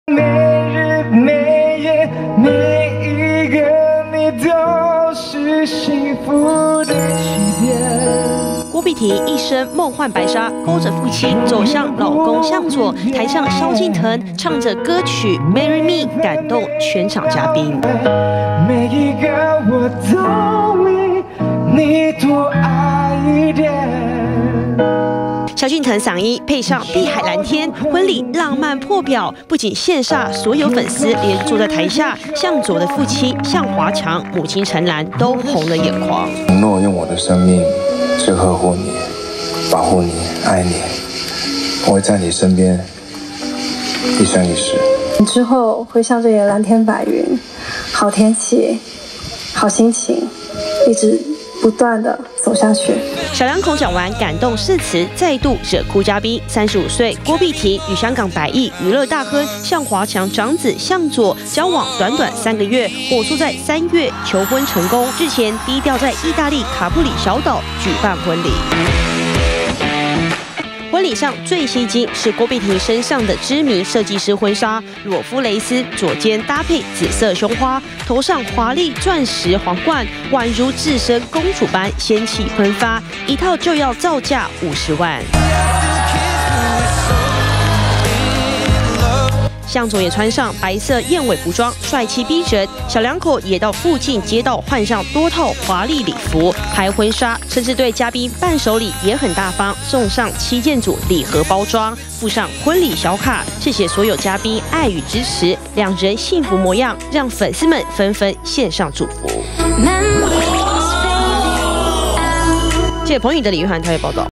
20190922中天新聞 郭碧婷、向佐義大利完婚 蕭敬騰獻唱逼哭嘉賓
郭碧婷與香港百億娛樂大亨之子，向華強長子「向佐」，交往三個月，迅速墜入愛河，日前小倆口，已經到義大利舉辦婚禮，還找來蕭敬騰浪漫獻唱，逼哭不少嘉賓。